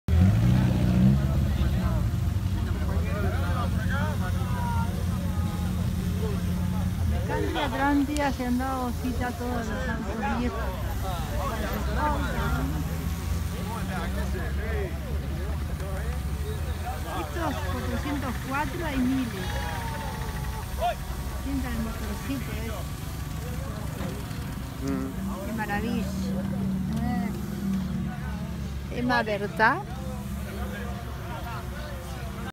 Atlantida Rally 19 capitales
llegada rally 2017 autos IMG 0697 al Country Club de  Atlántida x EB febrero . Uruguay ,  autos  argentinos y uruguayos  .